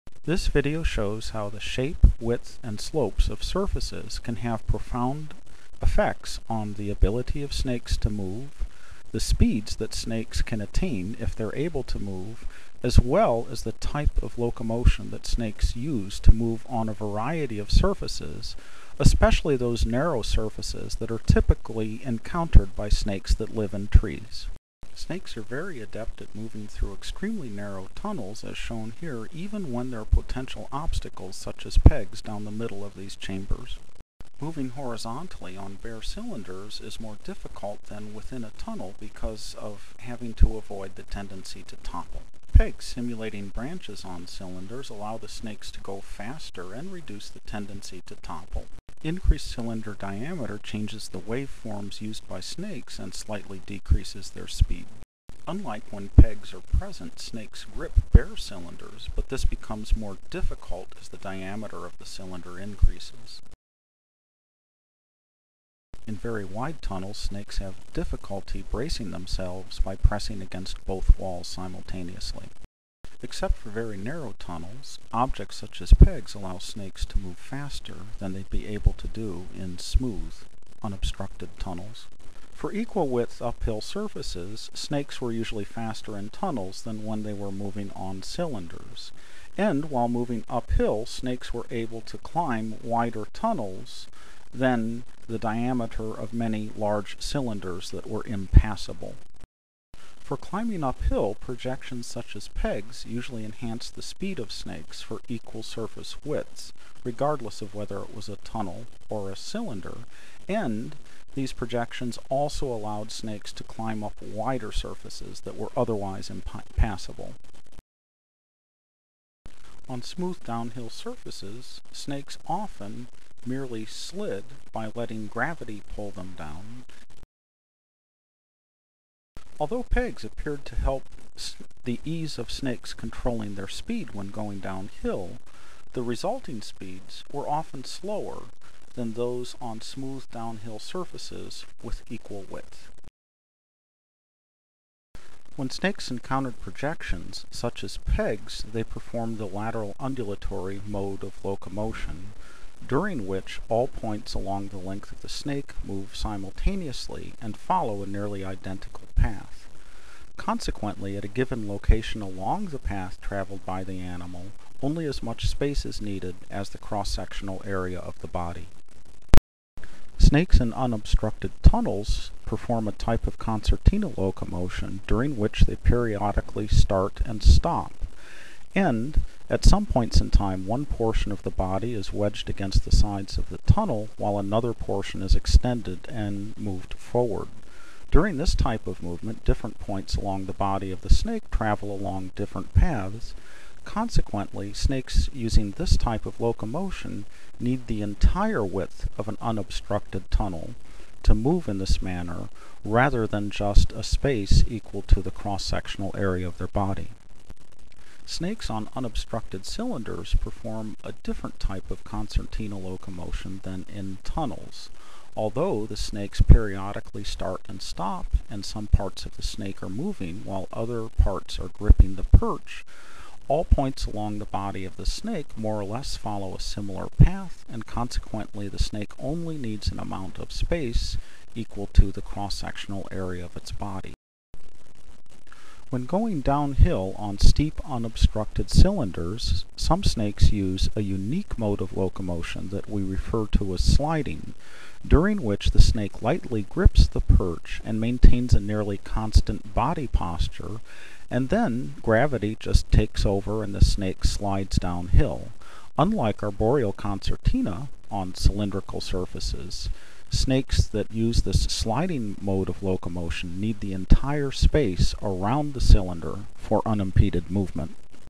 How snakes move: Effects of surface width, incline and surface structure on corn snake locomotion
See how corn snakes move depending on the width, shape and slope of surfaces